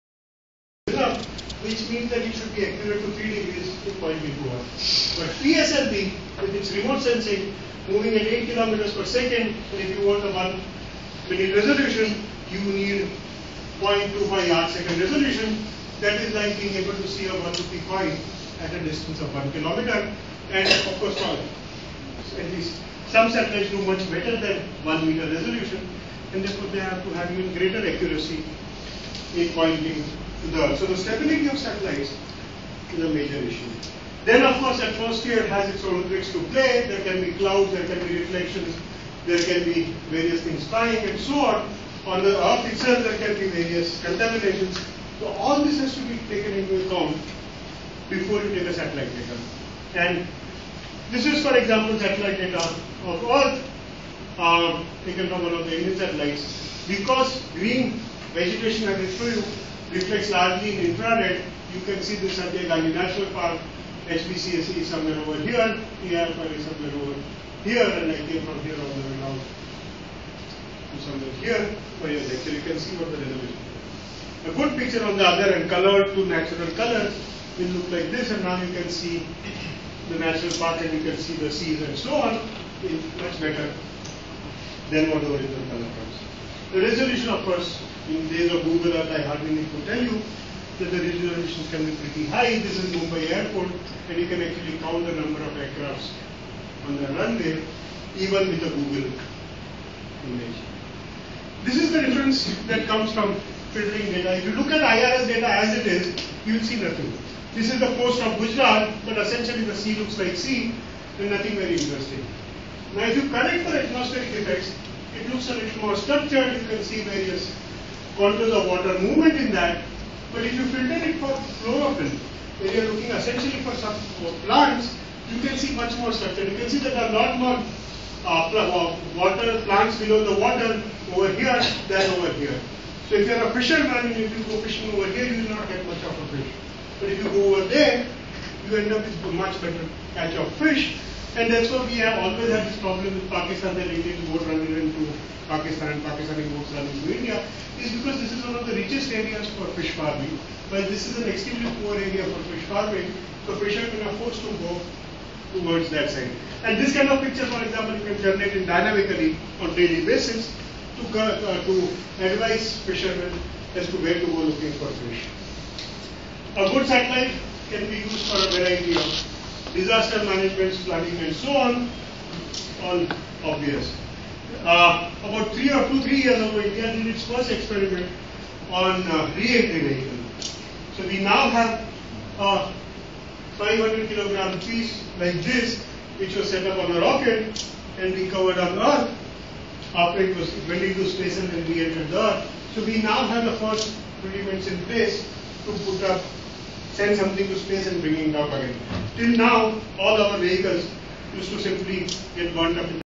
Space mission & Technology in India
Prof Mayank N Vahia gave a lecture on Space Mission and Technology involved in it in Indian context on 3 November 2015. It is a short clip of that nearly 1hr lecture.
Place: HBCSE,Mumbai
Recorded with Samsung Galaxy Fit